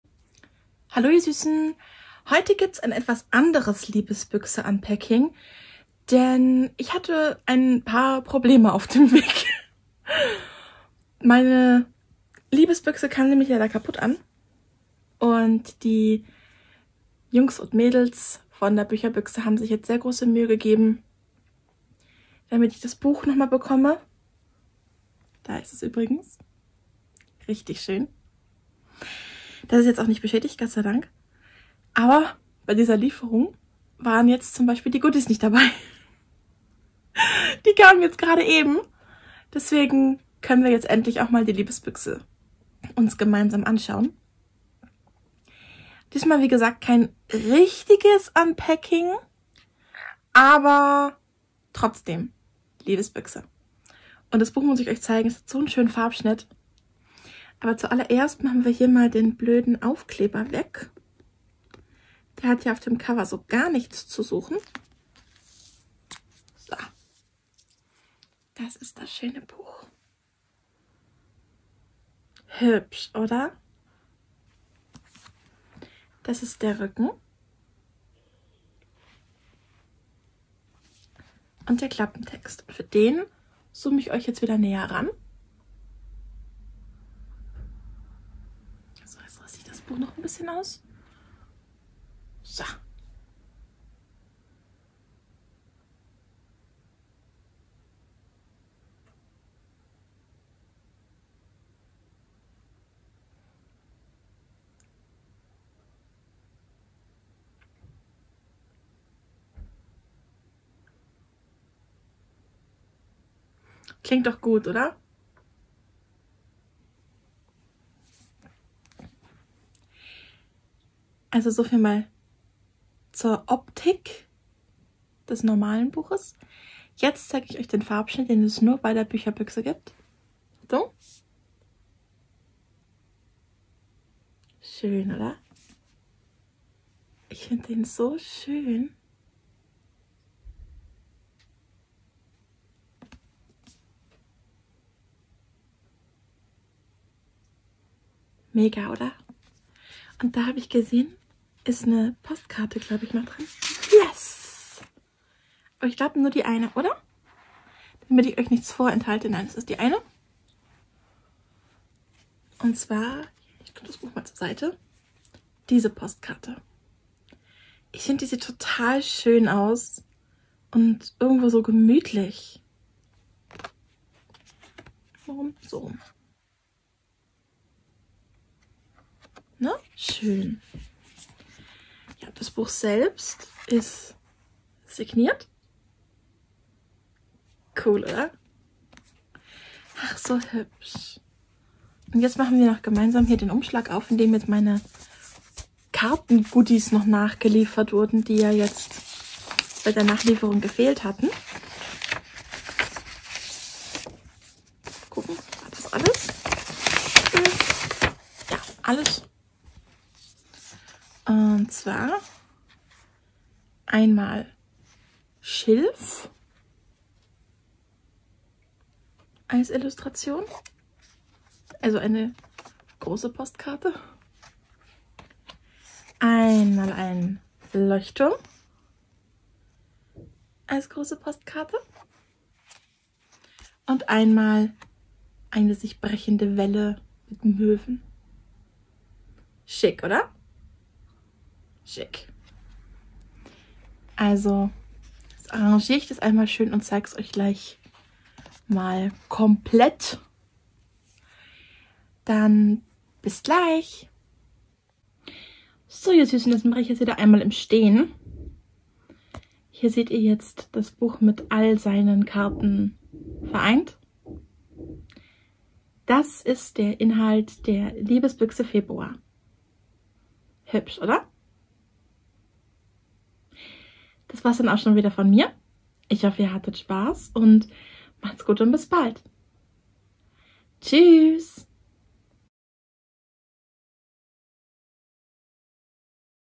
Unpacking: Liebesbüchse Februar 2023
Dieses Mal mit einigen Hindernissen ...

Ihr könnt euch den Inhalt noch einmal in Ruhe auf meinem Blog anschauen:

Über die Box:

Anbieter:   Bücherbüchse

Titel:   Liebesbüchse Februar

Monat:   Februar

Abo / Einzelkauf:   Abo (aber auch als Einzelkauf verfügbar)

Farbschnitt:   Ja

Preis:  16,99€

Link:   verfügbar

Beschreibung / Klappentext:

Was dich in der Februar Liebesbüchse erwartet?

Den Wind um die Nase, die Sonne im Gesicht und das Schaukeln des Segelschiffs auf den Wellen – was will man mehr? Doch genau da taucht plötzlich ihr ehemals bester Freund und heimlicher Schwarm auf und muss mit ihr zusammen an einem neuen Projekt arbeiten. Dabei wird schnell klar, dass er nichts mehr mit ihr oder ihrer Heimat zu tun haben will. Ob das gut gehen kann?

Dieses Buch ist eine SIGNIERTE EXKLUSIVAUSGABE als Klappenbroschur mit digitalem FARBSCHNITT designed von @francis.eden_art.

Lieferzeit: Ende Februar